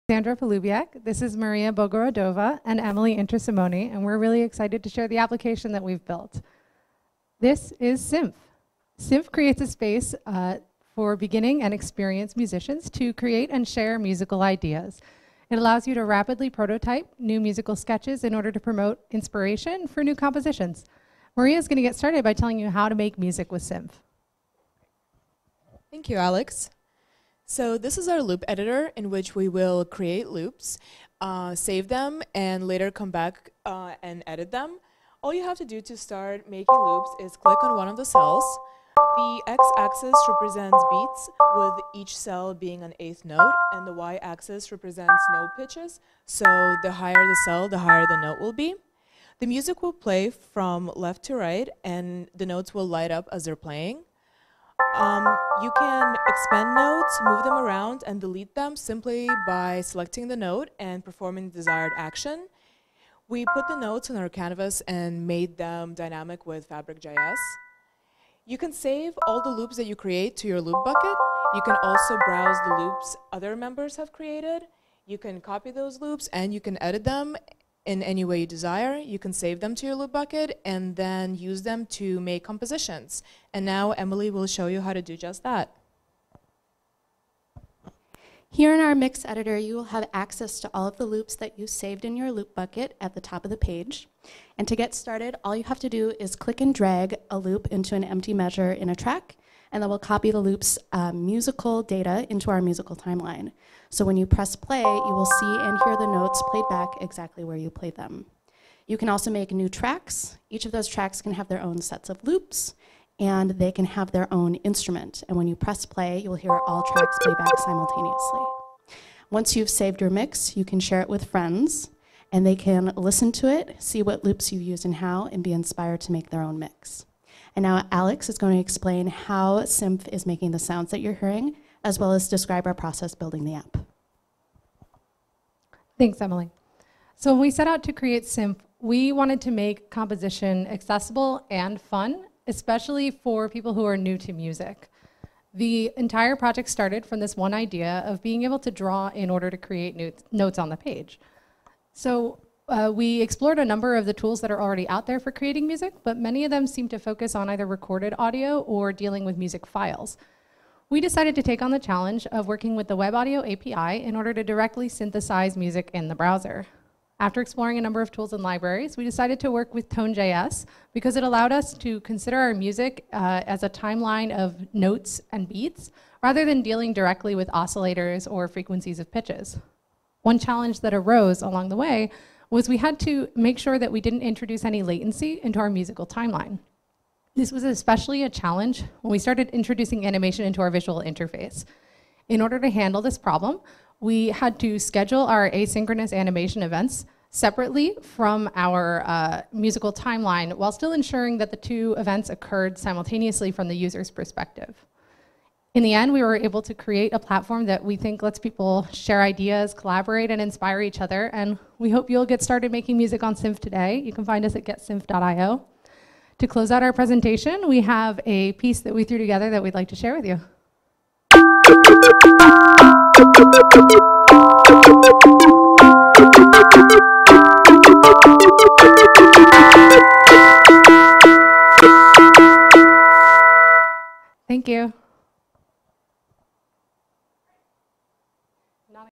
Grace Hopper Academy Capstone Project: Symph
Symph provides a space for both experienced and new musicians to create and share musical ideas. Users create bite-sized musical loops using a custom visual interface, built with FabricJS on an HTML5 canvas. Music is synthesized directly in the browser using ToneJS, an abstraction over the Web Audio API, allowing users to listen while they compose in real time.

Each User has a Loop Bucket, containing all of the Loops they’ve created. They can also browse other user-created loops, and save them into their Loop Bucket for later use. They can then use the Loops in their Loop Bucket to create a multi-track composition, by dragging and dropping loops into our Mix Editor. The Mix Editor was made using custom Angular Directives that utilize the HTML5 native Drag and Drop API.

After completing a Mix, you can save it, and it will be visible on the website for friends and other users to find. You can also browse mixes created by other users to get inspiration for your next composition.

Grace Hopper Academy is the world's leading engineering school for women, with no upfront tuition cost.